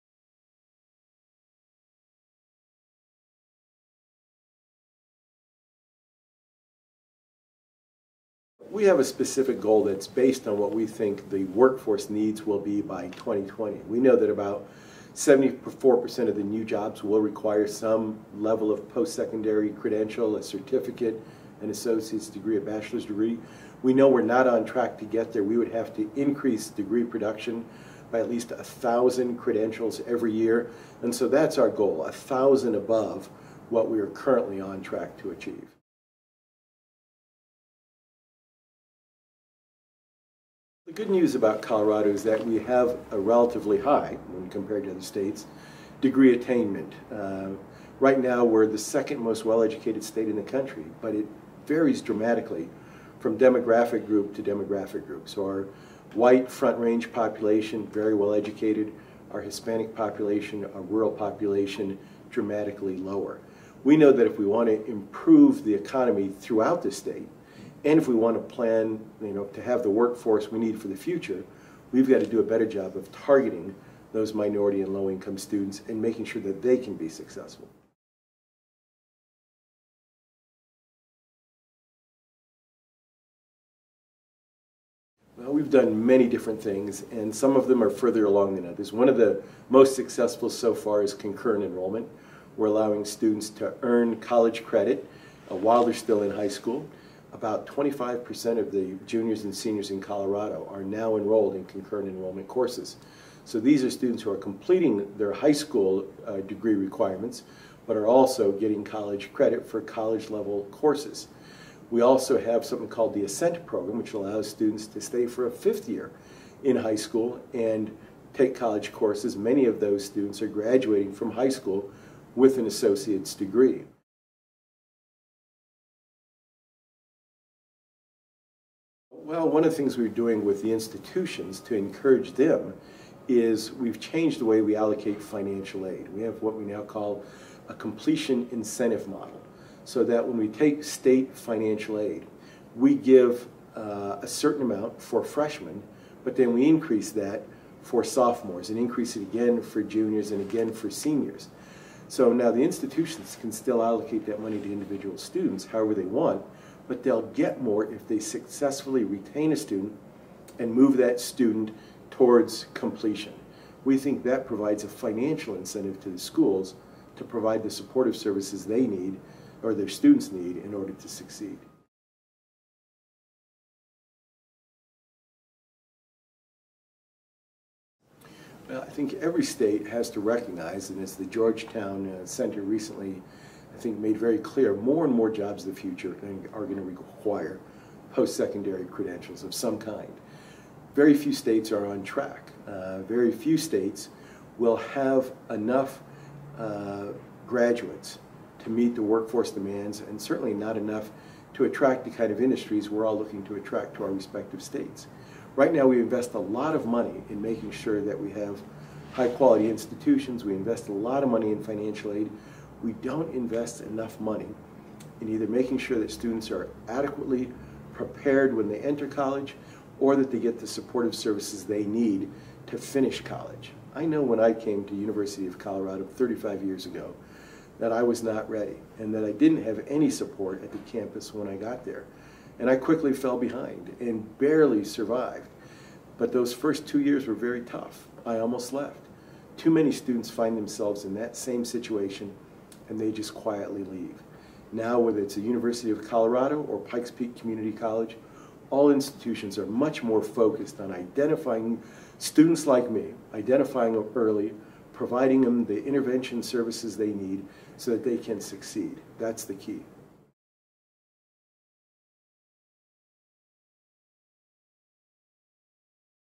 What is Colorado's attainment goal? Lt. Governor Joseph Garcia
Learn more about state policy for achieving America's postsecondary attainment goals